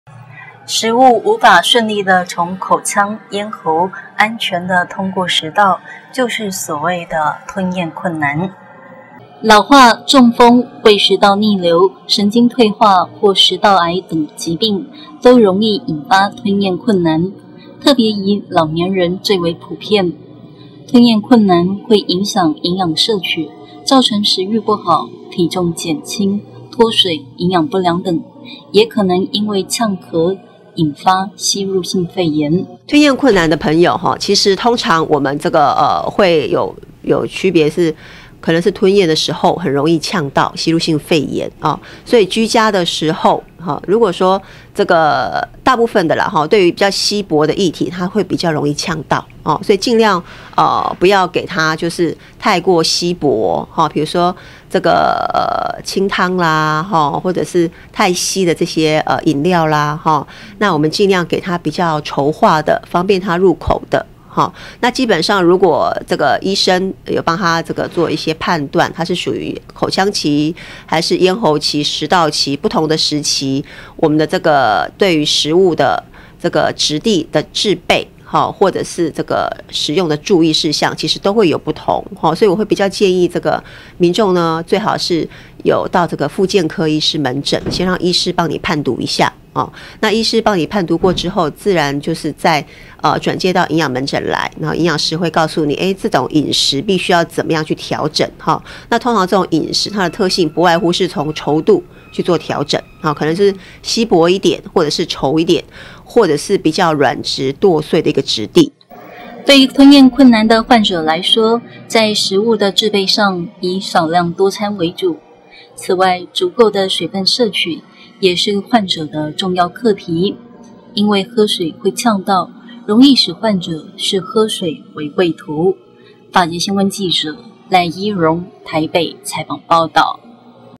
法界新聞》20140818吞嚥困難患者的飲食原則
法界新聞》
受訪者 ： 衛生福利部基隆醫院營養室主任  張皇瑜

老化、中風、胃食道逆流、神經退化或食道癌等疾病，都容易引發吞嚥困難，特別以老年人最為普遍。吞嚥困難會影響營養攝取，造成食慾不好、體重減輕、脫水、營養不良等，也可能因為嗆咳，引起吸入性肺炎。
對吞嚥困難的患者來說，在食物的製備上，以少量多餐為主。此外，足夠水分的攝取，也是吞嚥困難患者的重要課題，因為喝水會嗆到，容易使患者視喝水為畏途。